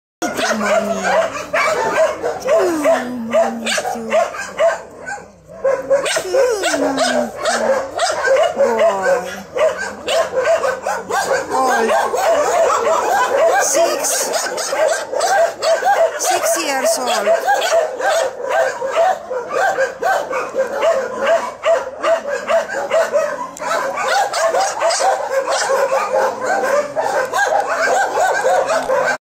Pedro in Rumänien
Pedro wurde von einem anderen Tierschutzverein übernommen und durfte nach Schweden ausreisen.